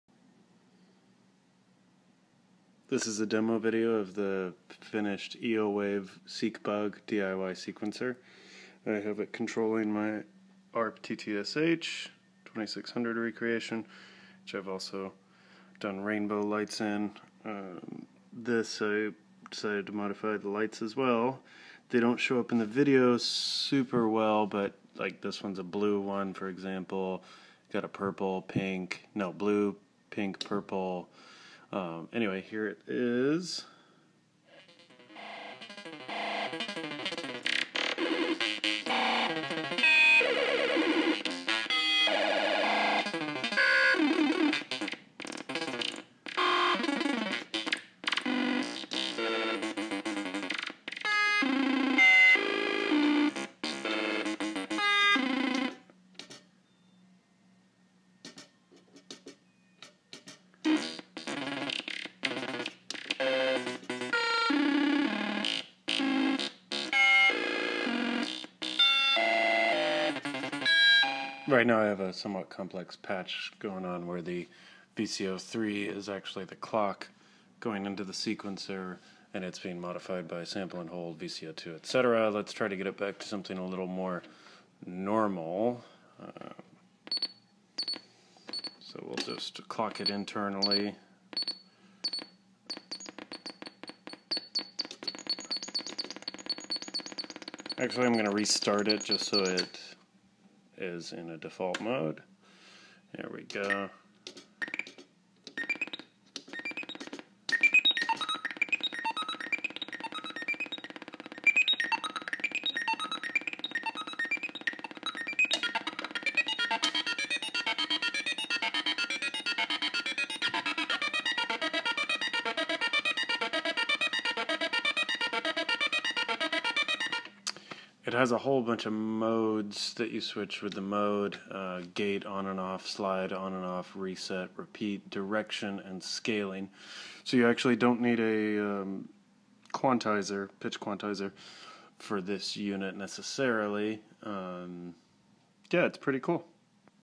eowave seq bug diy sequencer demo
not a very thorough demo, but just showing her off.